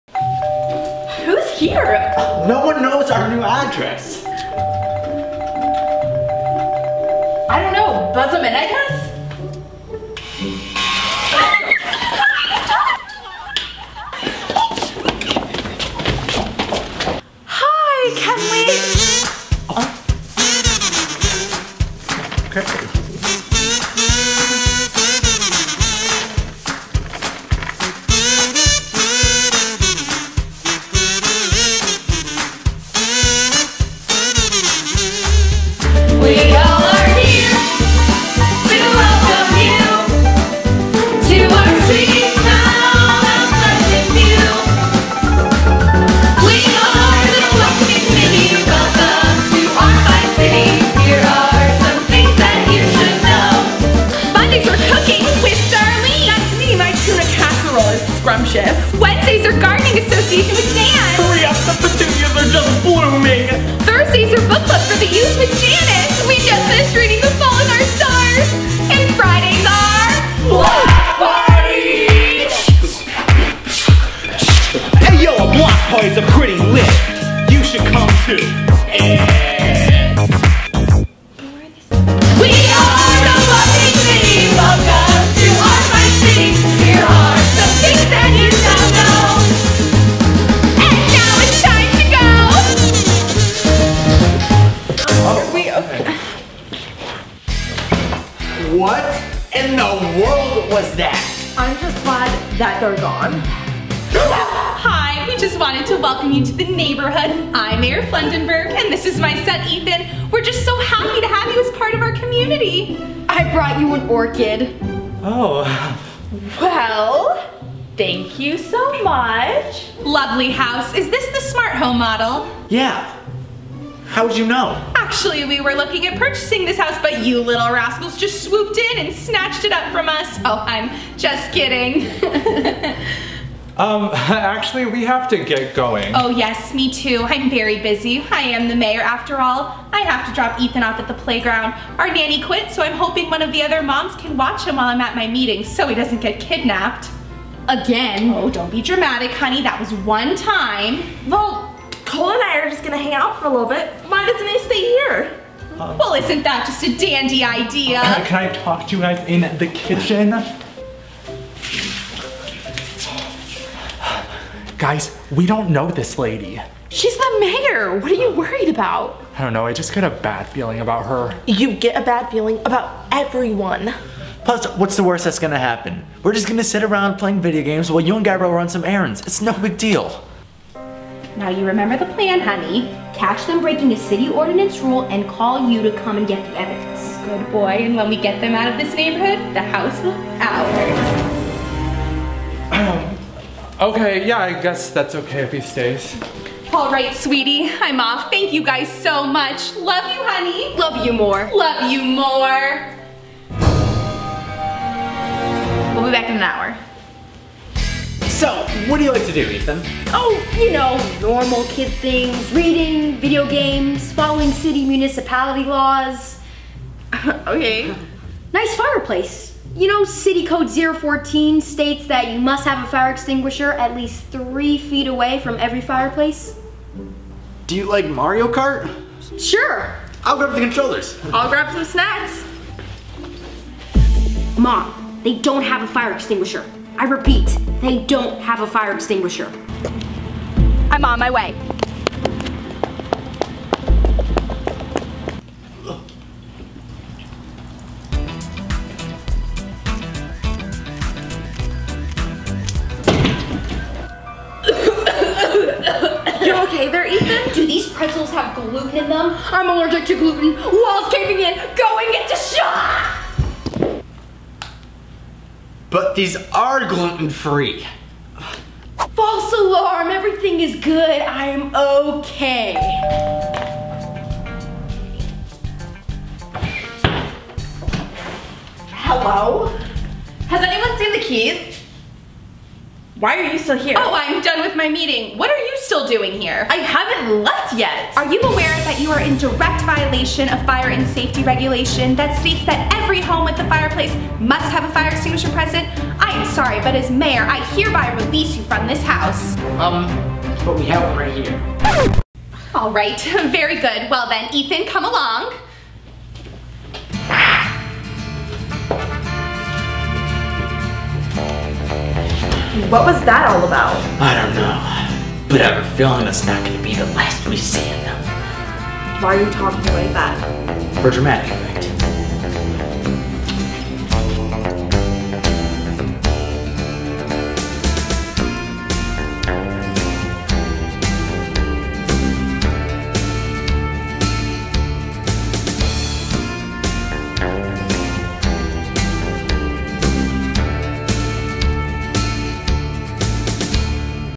Meeting our creepy new neighbors!
Season 4 - Episode 2: The neighborhood welcoming committee stops by the Skory's to welcome them the neighborhood, however there's something about the mayor and her son that doesn't seem entirely sincere...

Max: Max

 ⬇︎▿⬇︎▿⬇︎ More fun stuff down here ⬇︎▿⬇︎▿⬇︎